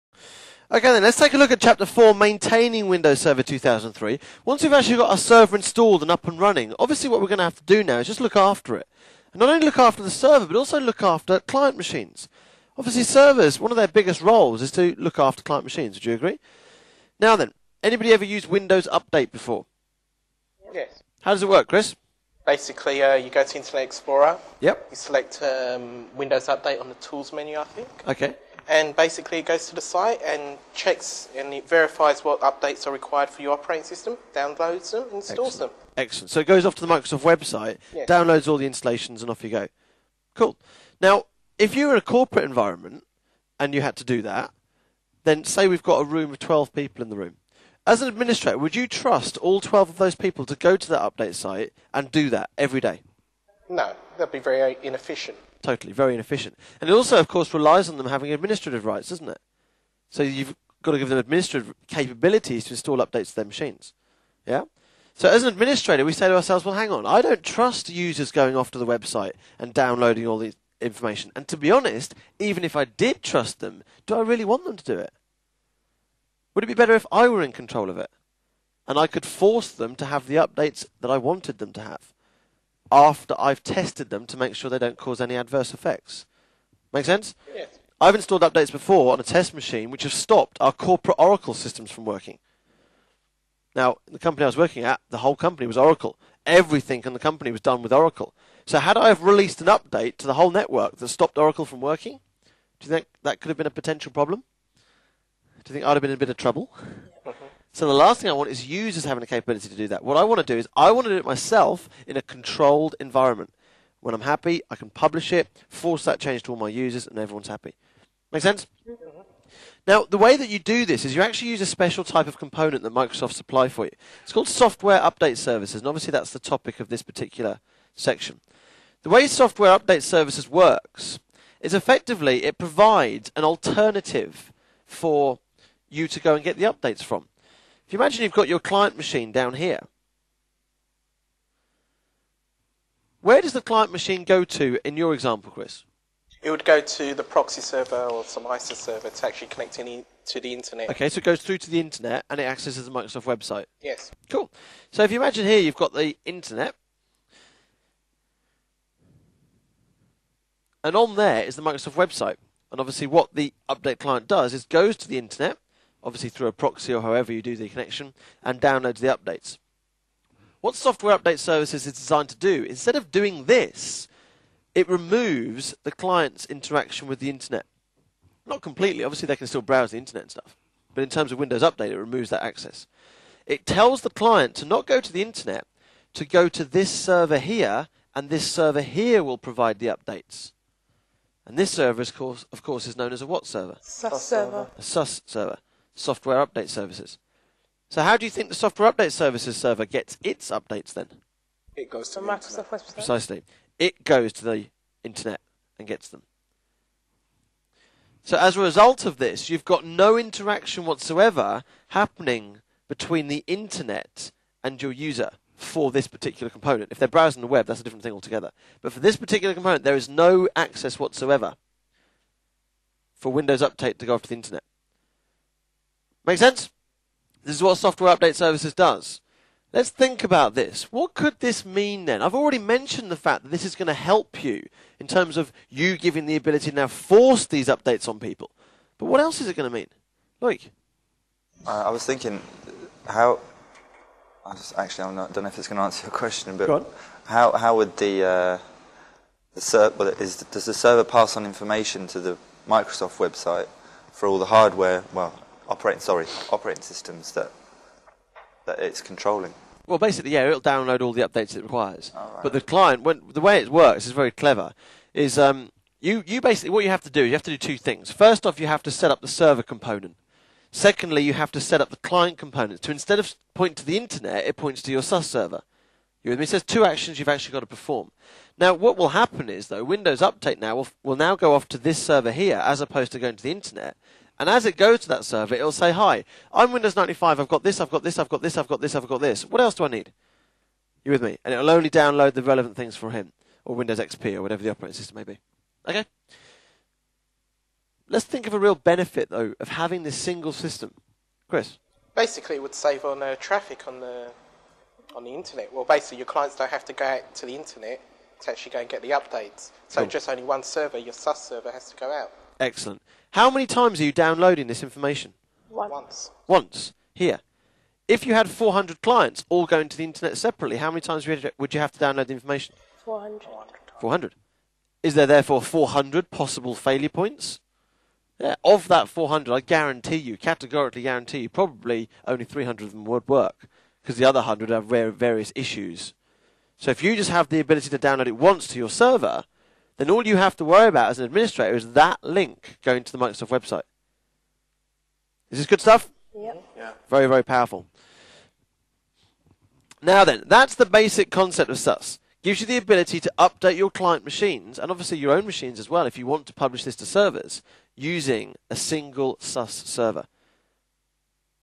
SmartLearn - Using Software Update Services (SUS) in Windows
Understanding and using Software Update Services (SUS) in Windows Server 2003. Very High Quality video shot in a style never before seen online.